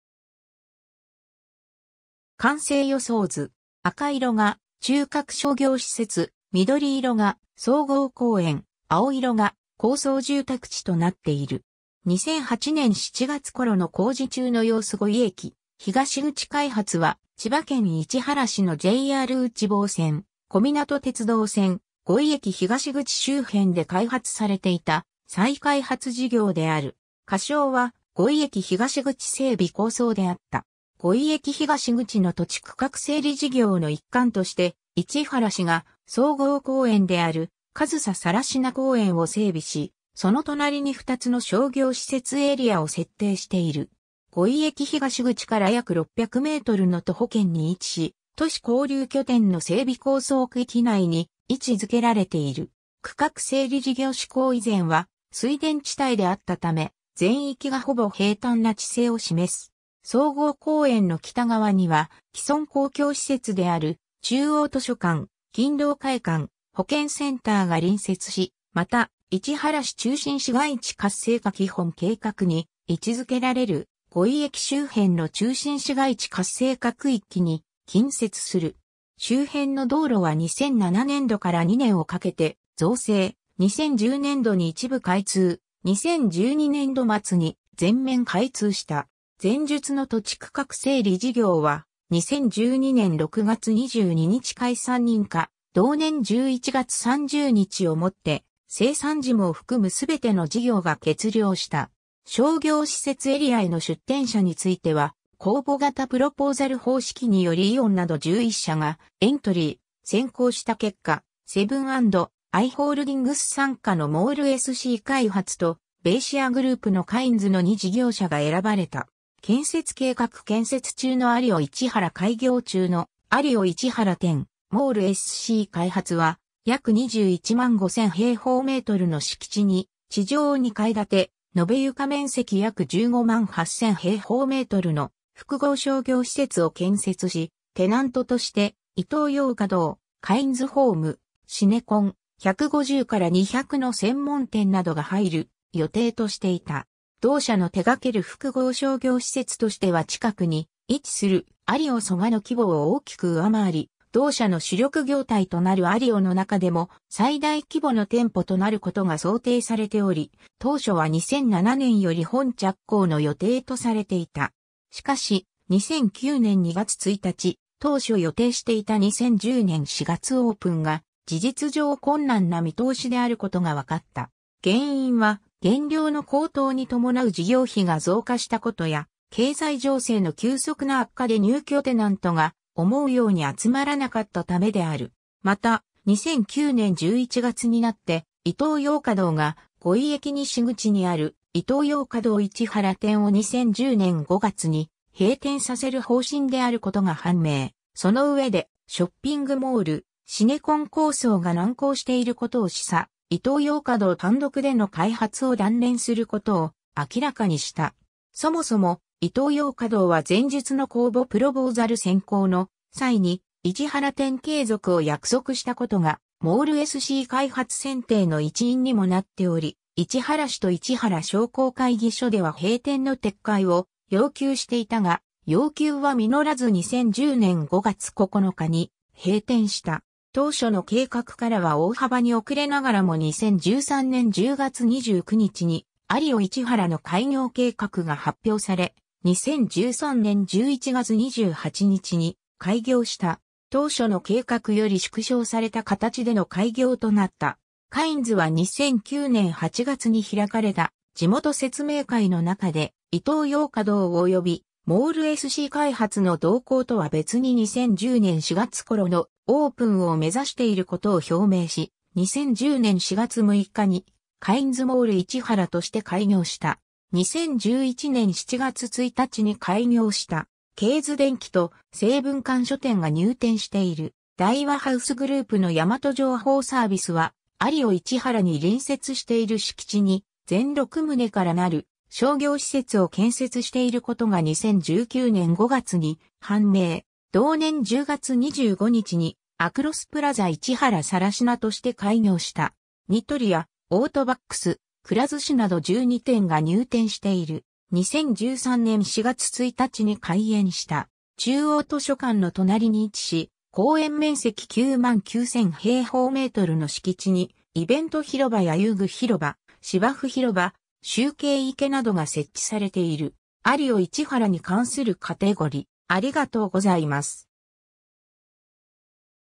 五井駅東口開発
完成予想図。
赤色が中核商業施設、緑色が総合公園、青色が高層住宅地となっている。
 2008年7月頃の工事中の様子 五井駅東口開発（ごいえきひがしぐちかいはつ）は、千葉県市原市のJR内房線・小湊鉄道線五井駅東口周辺で開発されていた再開発事業である。
仮称は五井駅東口整備構想であった。
 五井駅東口の土地区画整理事業の一環として、市原市が総合公園である「上総更級公園」を整備し、その隣に2つの商業施設エリアを設定している。
五井駅東口から約600mの徒歩圏に位置し、都市交流拠点の整備構想区域内に位置づけられている。
 区画整理事業施行以前は水田地帯であったため、全域がほぼ平坦な地勢を示す。
総合公園の北側には、既存公共施設である中央図書館、勤労会館、保健センターが隣接し、また「市原市中心市街地活性化基本計画（平成15年4月）」に位置づけられる五井駅周辺の中心市街地活性化区域に近接する。
周辺の道路は2007年度から2年を掛けて造成、2010年度に一部開通、2012年度末に全面開通した。
 前述の土地区画整理事業は、2012年6月22日解散認可、同年11月30日をもって、清算事務を含むすべての事業が結了した。
 商業施設エリアへの出店者については公募型プロポーザル方式によりイオンなど11社がエントリー、選考した結果、セブン&アイ・ホールディングス傘下のモール・エスシー開発（現 セブン&アイ・クリエイトリンク）と、ベイシアグループのカインズの2事業者が選ばれた。
 建設計画 建設中のアリオ市原 開業中のアリオ市原店(2017年10月9日撮影) モール・エスシー開発は約215,000m2の敷地に地上2階建て・延床面積約158,000m2の複合商業施設を建設し、テナントとしてイトーヨーカドー、カインズホーム、シネコン、150から200の専門店などが入る予定としていた。
同社の手がける複合商業施設としては近くに位置するアリオ蘇我の規模を大きく上回り、同社の主力業態となるアリオの中でも最大規模の店舗となることが想定されており、当初は2007年より本着工の予定とされていた。
 しかし、2009年2月1日、当初予定していた2010年4月オープンが事実上困難な見通しであることがわかった。
原因は原料の高騰に伴う事業費が増加したことや経済情勢の急速な悪化で入居テナントが思うように集まらなかったためである。
 また、2009年11月になって、イトーヨーカ堂が五井駅西口にある「イトーヨーカドー市原店」を2010年5月に閉店させる方針である事が判明、その上でショッピングモール、シネコン構想が難航していることを示唆、イトーヨーカ堂単独での開発を断念することを明らかにした。
そもそも、イトーヨーカ堂は前述の公募プロボーザル選考の際に市原店継続を約束したことがモール・エスシー開発選定の一因にもなっており、市原市と市原商工会議所では閉店の撤回を要求していたが、要求は実らず2010年5月9日に閉店した。
 当初の計画からは大幅に遅れながらも2013年10月29日に、「アリオ市原」の開業計画が発表され、2013年11月28日に開業した。
当初の計画より縮小された形での開業となった。
 カインズは2009年8月に開かれた地元説明会の中で、イトーヨーカ堂およびモール・エスシー開発の動向とは別に2010年4月頃のオープンを目指している事を表明し、2010年4月6日に「カインズモール市原」として開業した。
 2011年7月1日に開業した。
ケーズデンキと精文館書店が入店している。
 大和ハウスグループの大和情報サービスはアリオ市原に隣接している敷地（これまでは臨時駐車場として利用）に全6棟からなる商業施設を建設している事が2019年5月に判明。
同年10月25日に「アクロスプラザ市原更級」として開業した。
ニトリやオートバックス、くら寿司など12店が入店している。
 2013年4月1日に開園した。
中央図書館の隣に位置し、公園面積99,000m2の敷地にイベント広場や遊具広場、芝生広場、修景池などが設置されている。
 アリオ市原に関するカテゴリ: